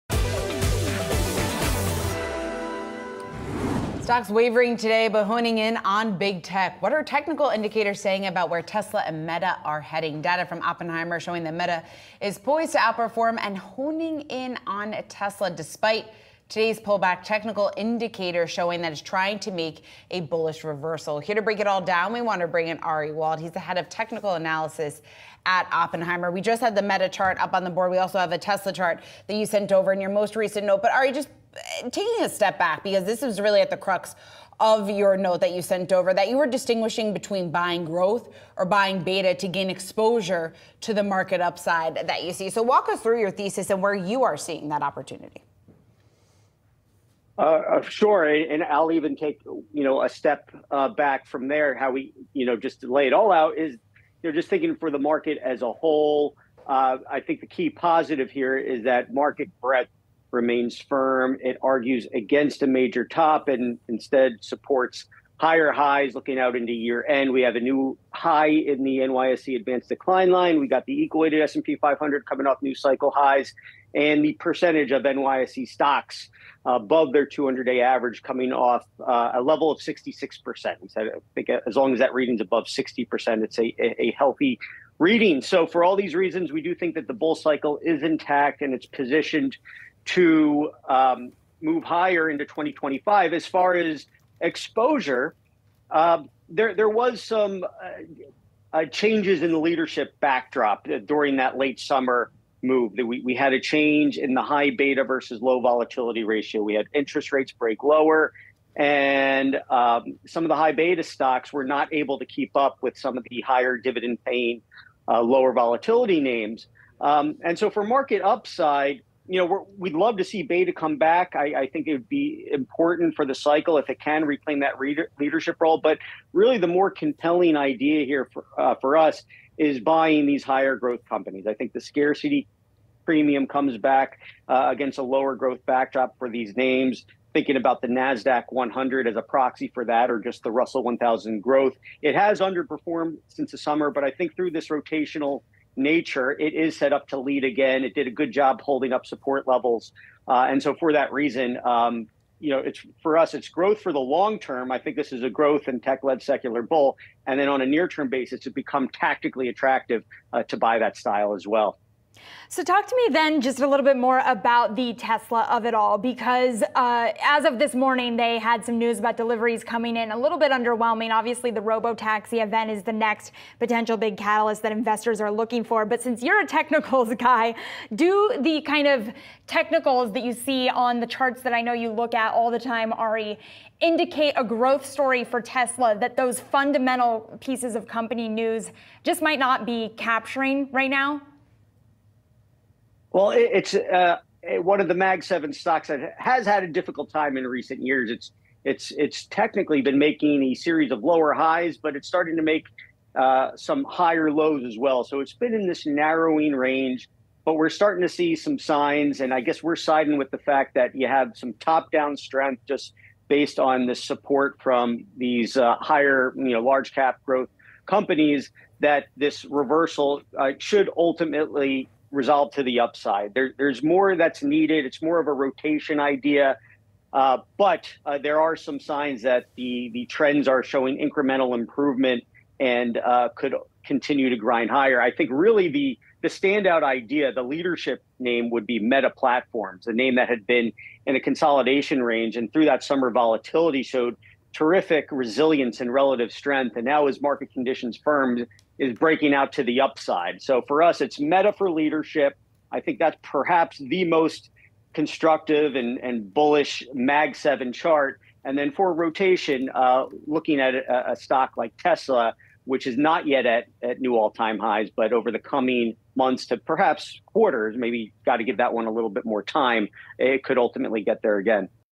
Why the Nasdaq is positioned for long-term growth
Technical indicators from Oppenheimer suggest that big tech companies, especially names like Meta (META) and Tesla (TSLA), are poised to outperform in the coming months. Oppenheimer Head of Technical Analysis Ari Wald joins Catalysts to discuss this outlook. Wald emphasizes that the key positive for markets is that market breadth "remains firm," supporting higher highs throughout the end of 2024. Based on this, he believes "the bull cycle is intact," with the market positioned for continued growth into 2025. He advises buying higher growth companies, explaining, "The scarcity premium comes back against a lower growth backdrop for these names," especially within the tech-heavy Nasdaq (^IXIC). Wald elaborates on the Nasdaq's potential: "It has underperformed since the summer, but I think through this rotational nature it is set up to lead again. It did a good job holding up support levels and so for that reason, for us, it's growth for the long-term. I think this is a growth and tech-led secular bull, and then on a near-term basis, it's become tactically attractive to buy that style as well."
About Yahoo Finance: